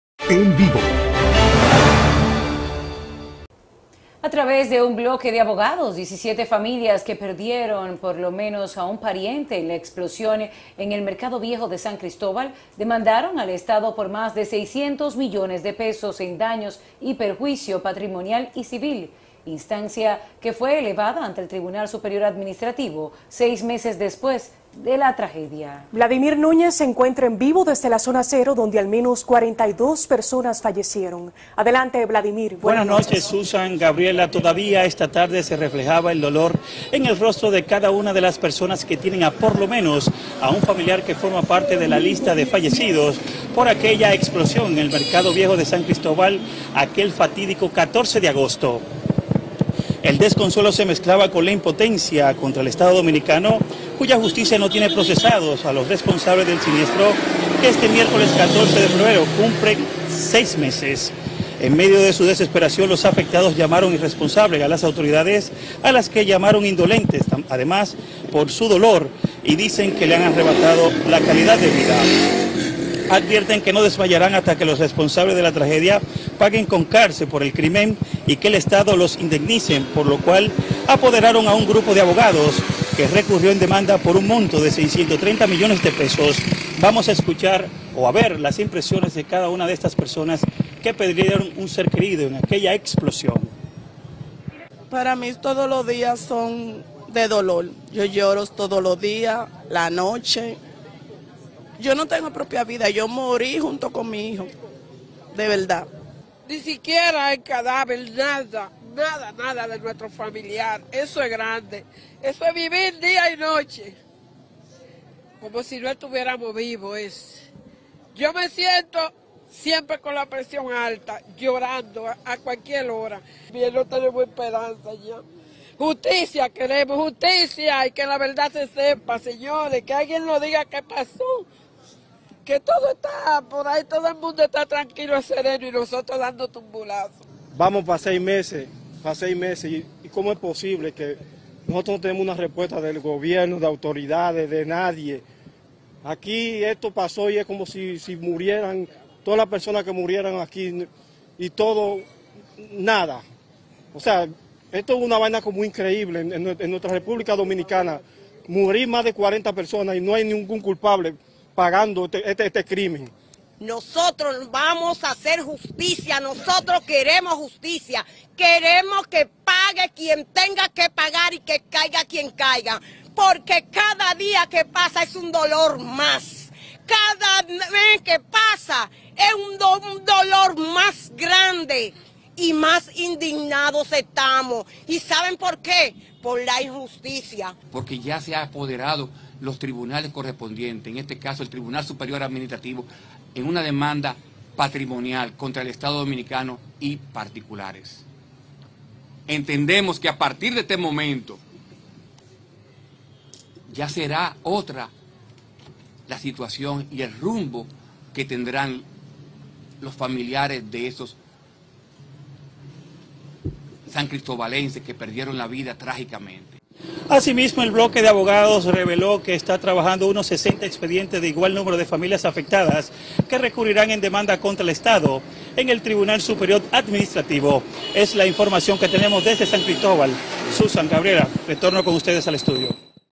Al cumplirse 6 meses de la explosión en San Cristóbal, deudos de las víctimas esperan por justicia
A seis meses de la explosión en San Cristóbal, el dolor se refleja en el rostro de cada una de las personas que perdieron un familiar que forma en el suceso.

En medio de su desesperación los afectados llamaron “irresponsable” a las autoridades a las que llamaron indolentes, además por su dolor dicen que le han arrebatado la calidad de vida.